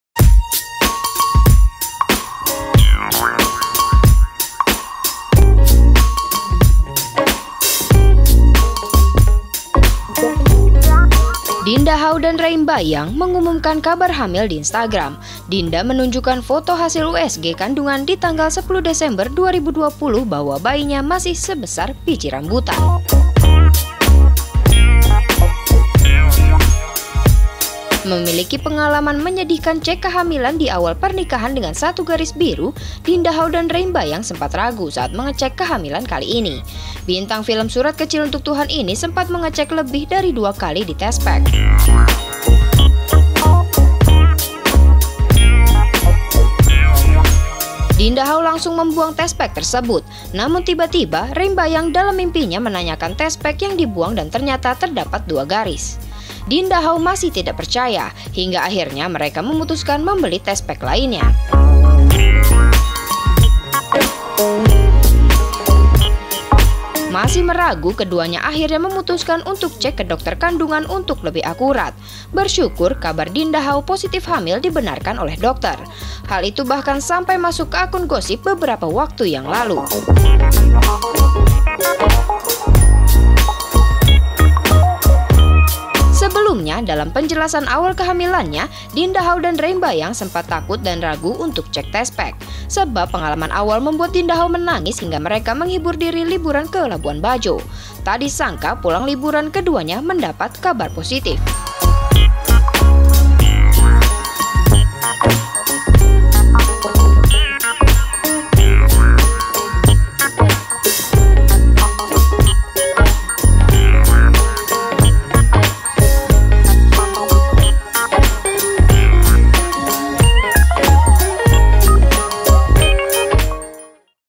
Dinda Hauw Hamil
dinda hauw,
rey mbayang,
hamil 11minggu,

Dinda Hauw dan Rey Mbayang mengumumkan kabar hamil di Instagram. Dinda menunjukkan foto hasil USG kandungan di tanggal 10 Desember 2020 bahwa bayinya masih sebesar biji rambutan.